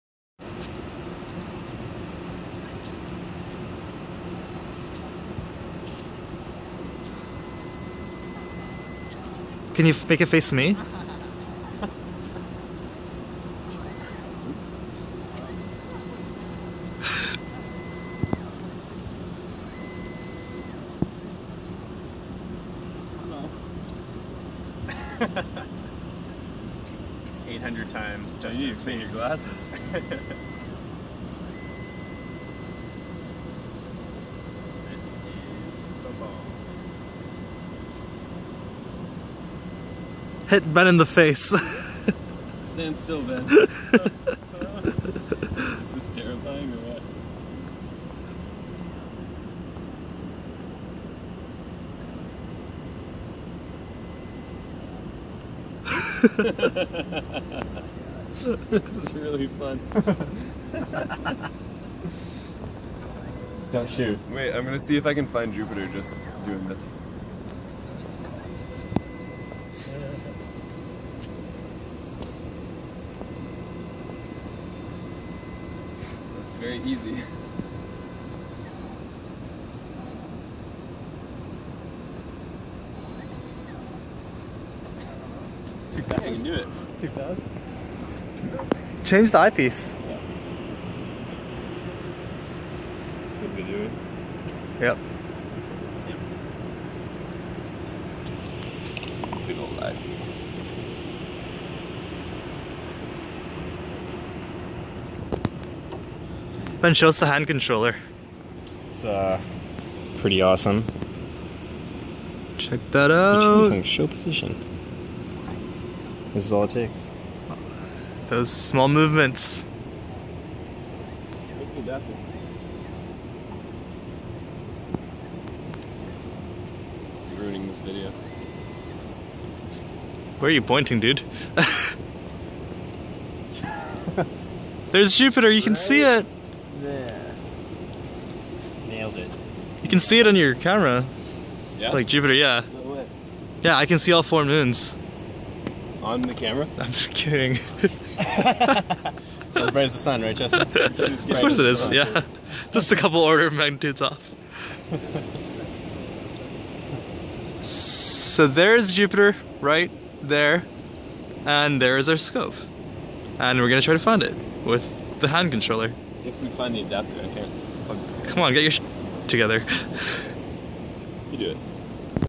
Goofing around with our skywatcher EQ8 and a 16"
The UBC Astronomy Club and our new skywatcher EQ8 mount. We put our 16" RCT on it and took a look at Jupiter.